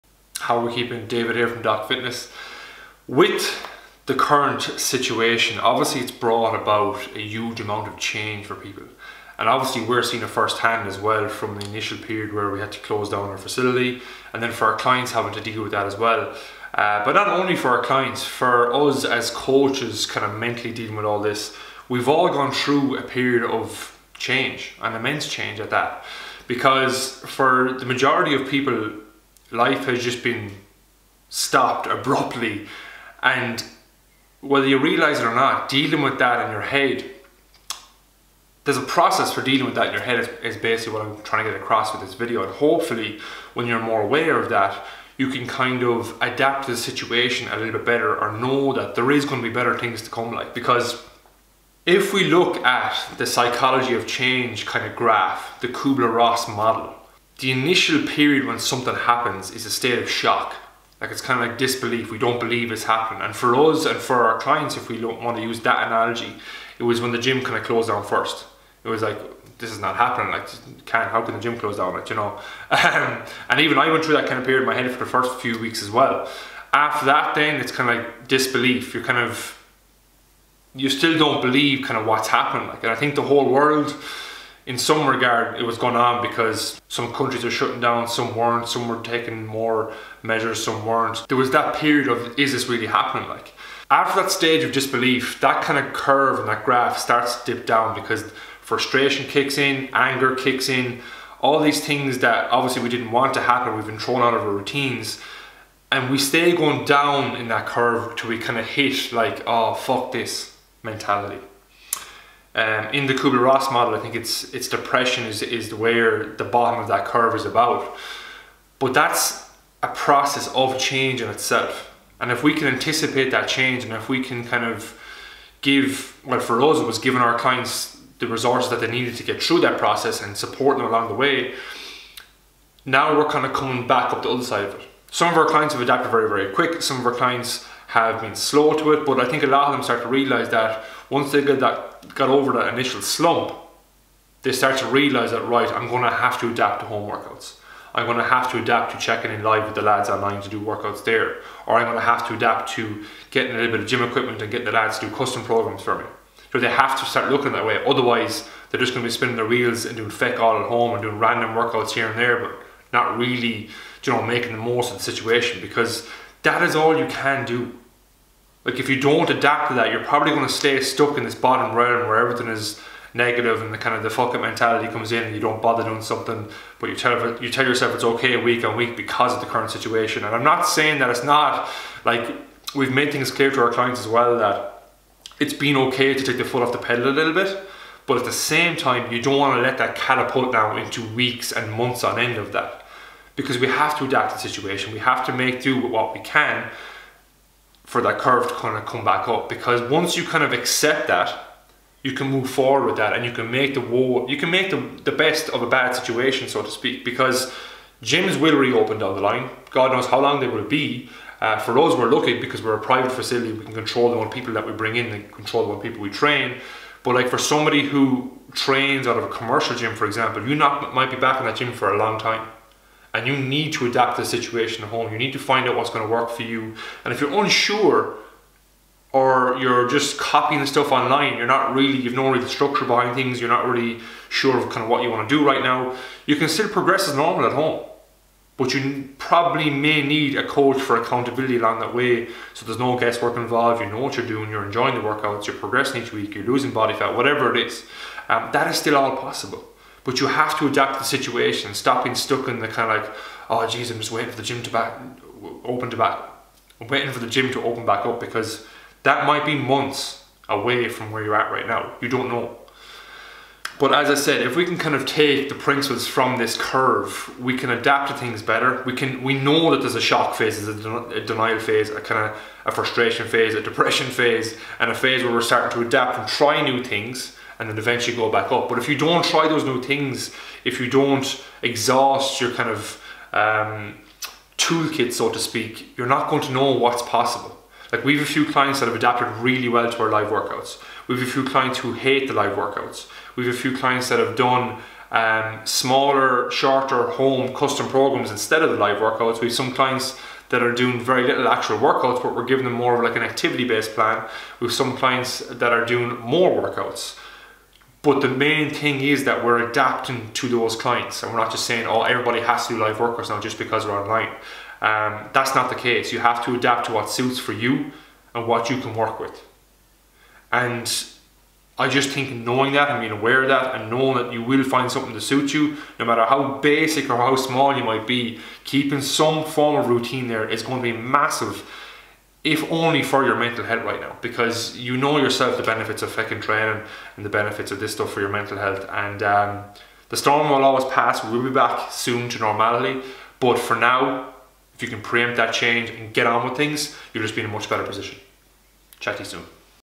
Changing with Covid
With routines being thrown out of whack, gyms closed and motivation all over the place; there's a certain element of change at play that you probably need to be aware of so you can adapt to the situation as best you can.

The 'psychology' of change is something the whole World is going through right now and here's what that means for you, your health and your fitness.

Or, if you're Looking to find out more about our coaching services? Go here;